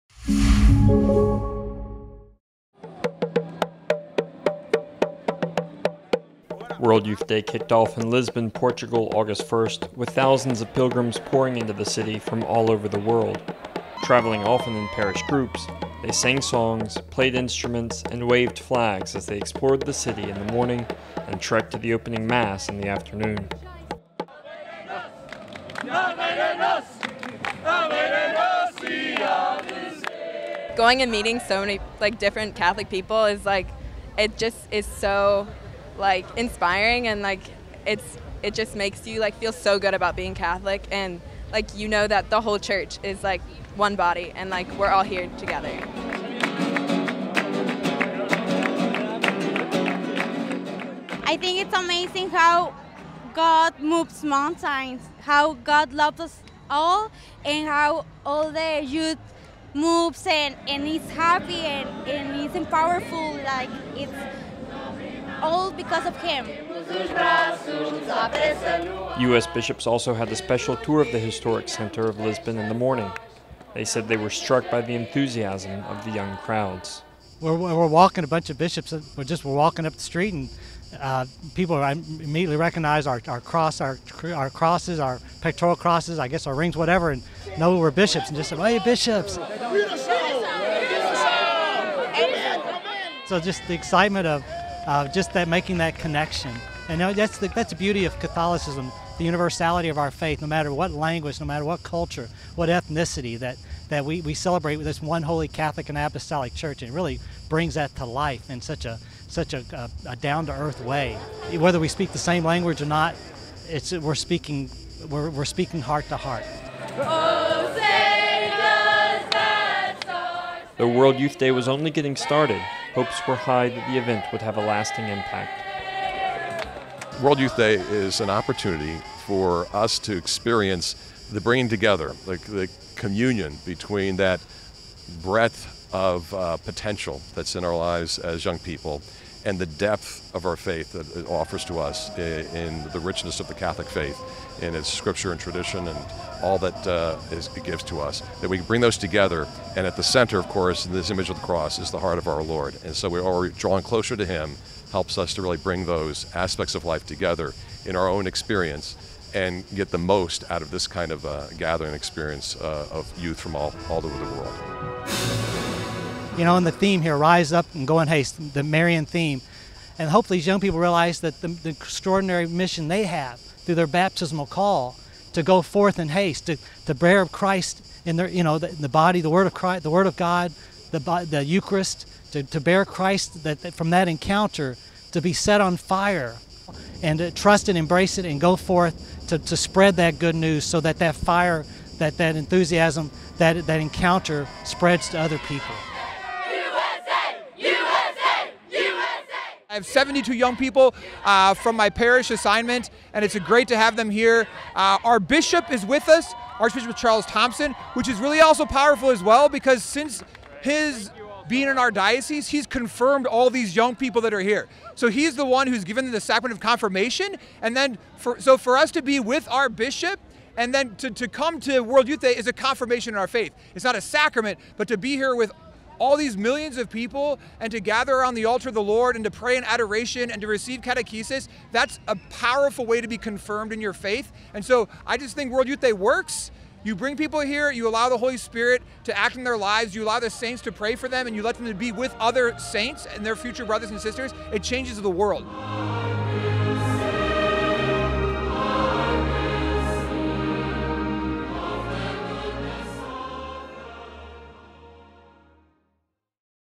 WYD kicks off in Lisbon
World Youth Day kicked off in Lisbon, Portugal Aug. 1 with thousands of pilgrims pouring into the city from all over the world.